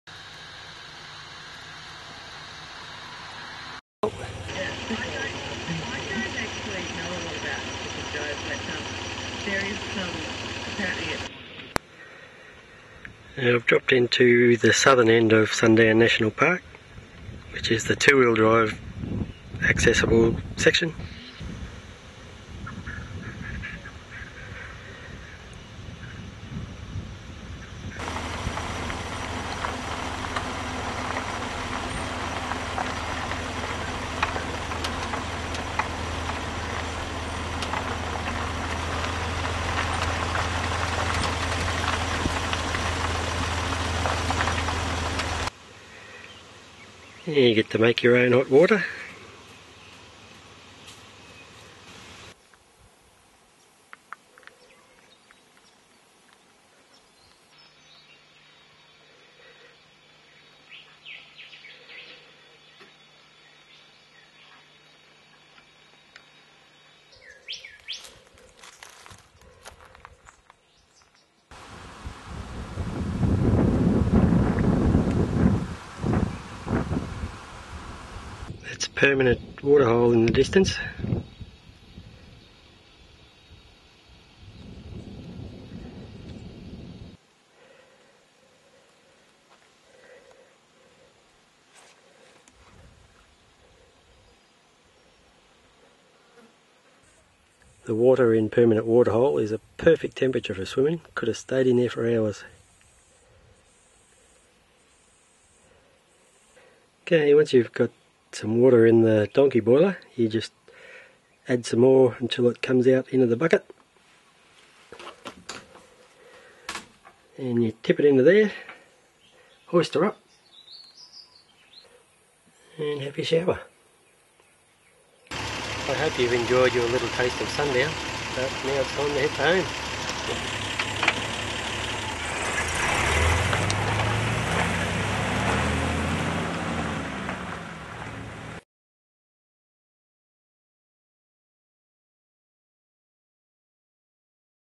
Sundown National Park part 2
Video of a trip to the 2WD section of Sundown National Park.